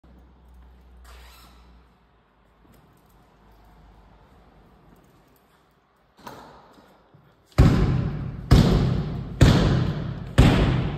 Bare knuckle boxing
In this boxing video, I'm going to show you how to do a left hook without gloves on the heavy bag. This is a great exercise for developing your punching power and accuracy.

If you're looking to add some punching power and accuracy to your training, then you need to watch this left hook without gloves video!